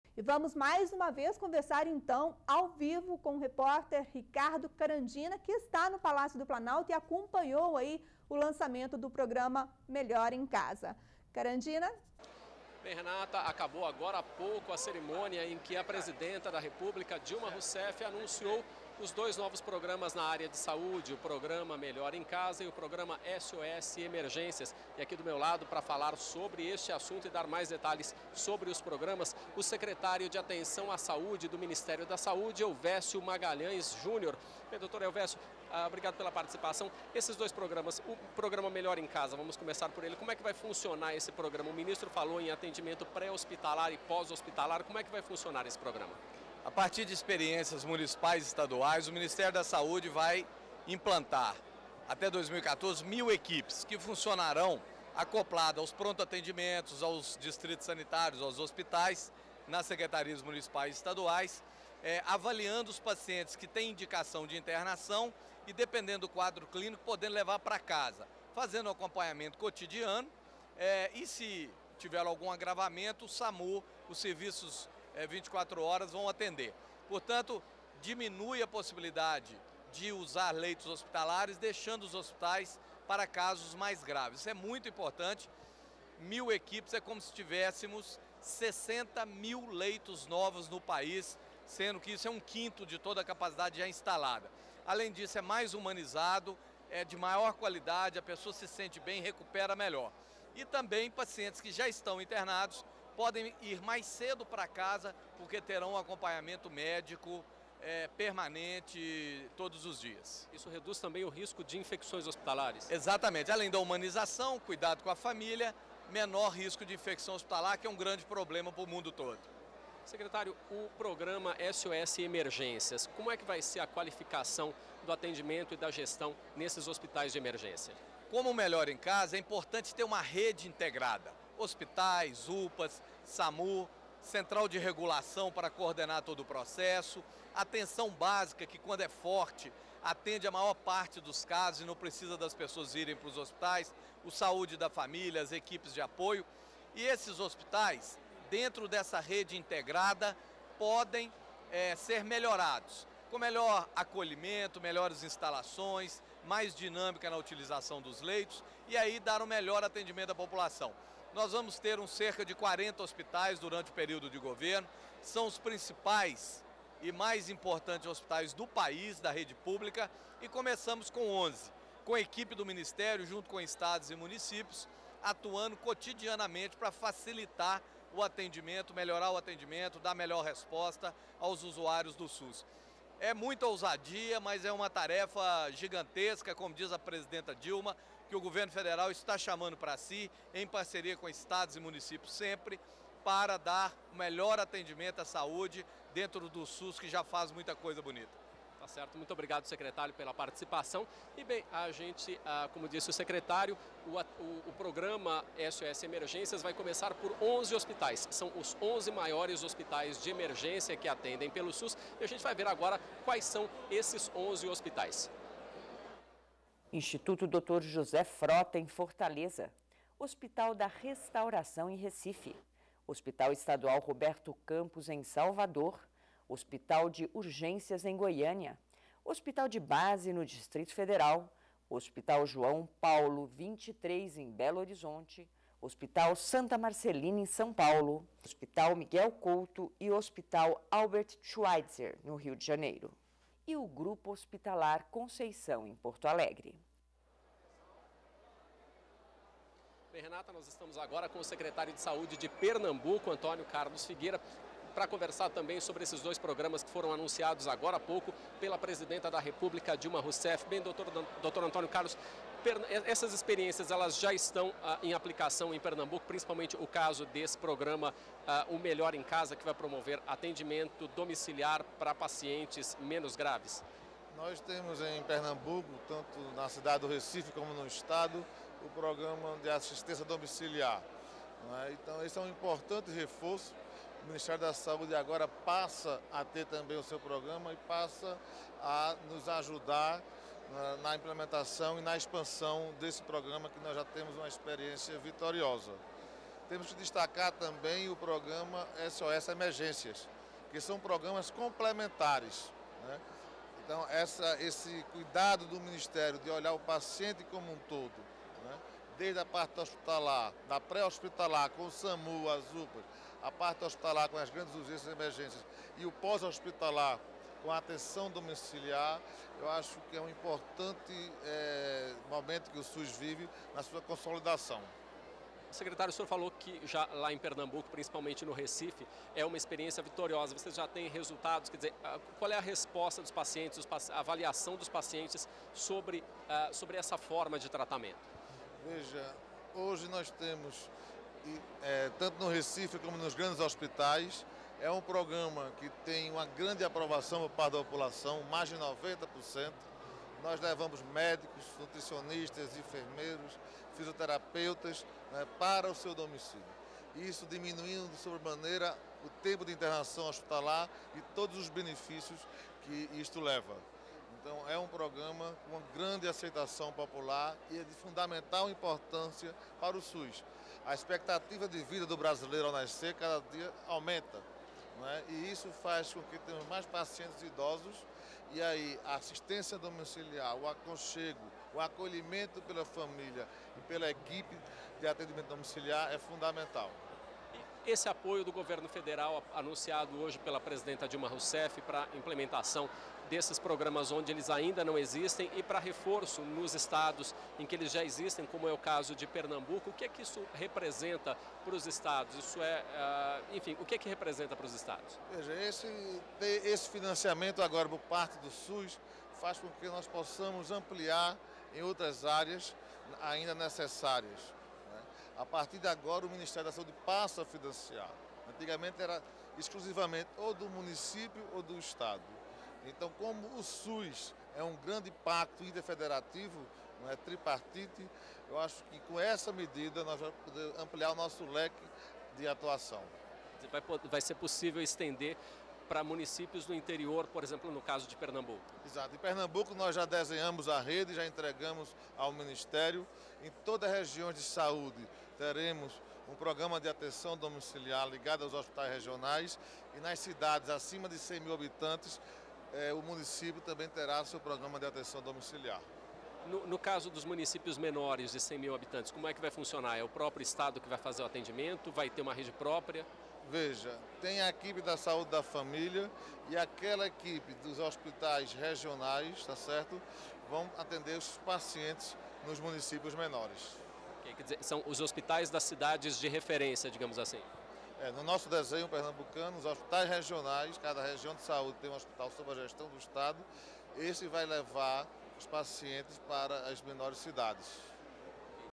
Secretário do MS explica como funcionará o "Melhor em Casa" e o "SOS Emergências"
Flash realizado após cerimônia de lançamento dos porgramas do Governo Federal, Melhor em Casa e SOS Emergências. O sercretário de Atenção å saúde, Elvécio Magalhães Júnior detalha como vai funcionar os programas.